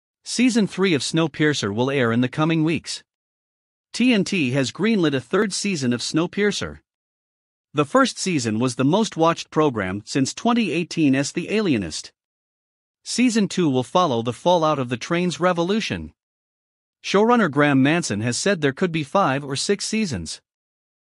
Season 3 of Snowpiercer will air in the coming weeks.
TNT has greenlit a third season of Snowpiercer. The first season was the most-watched program since 2018’s The Alienist. Season 2 will follow the fallout of the train’s revolution. Showrunner Graeme Manson has said there could be five or six seasons.